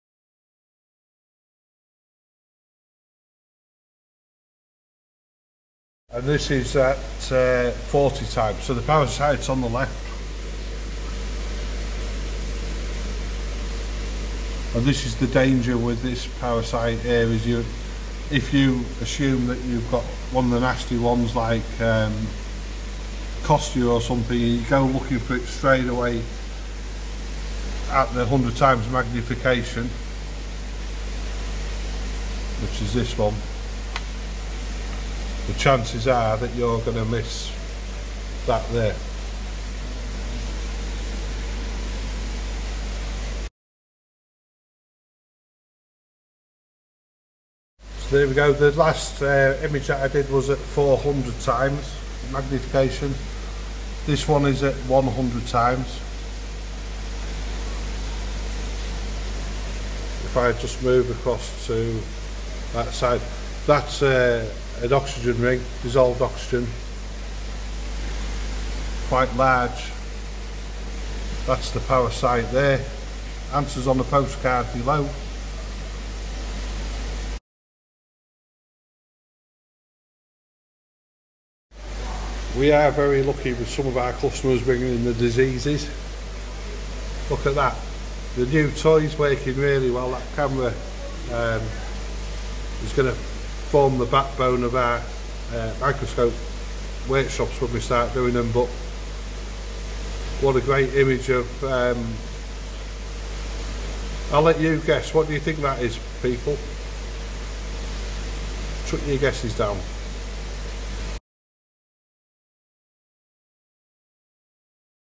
Elite Koi Fish Health - Koi Parasite Whitespot identified using a Microscope to identify parasites
Whitespot is a Fish Parasite and in the heirachy of Parasites with the ability to kill and do large damage to your Koi, It is one of the top!
Other Koi Parasites which we routinely identify using our Shop Microscope are Whitespot, Costia, Chillodonella (Chillo), Trichodina ( Trich), Skin Flukes and Gill Flukes.
If you suspect you have any of the top 3 Parasites, we would urge you to seek help in identifying and most importantly treating the pond to get rid of the parasites.
We have videoed the Whitespot Parasite at 40x, 100x and 400x magnification and very pleased with the images which we hope you will find useful in identifying the Koi Parasite.
@elitekoi offer a free scrape service for Grimsby Koi Keepers and Elite Koi customers by appointment only so please call in to see us if you need any help with sick Fish. Alternatively request us to give you a call when ordering any products from our